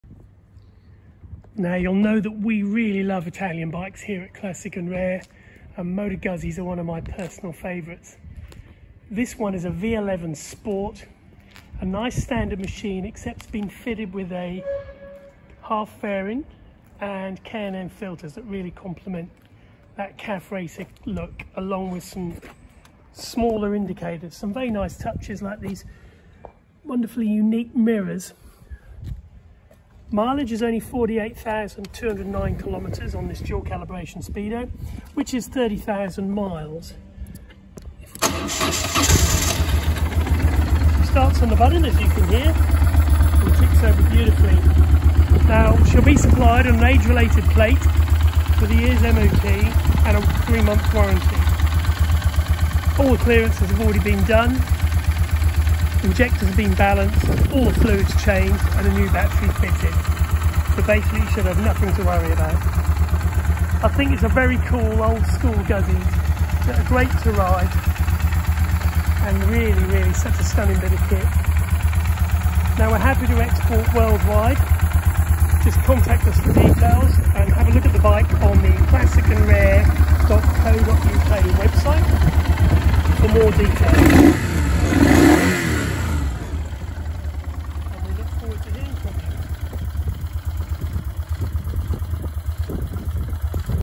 Moto Guzzi V11 Sport 182jb
Here we another stunning Moto Guzzi V11 Sport. This 2000 model is supplied registered on an age related plate with a full service.
A lovely example of this classic machine with K&N filters and a super cool half fairing to compliment that Café Racer look.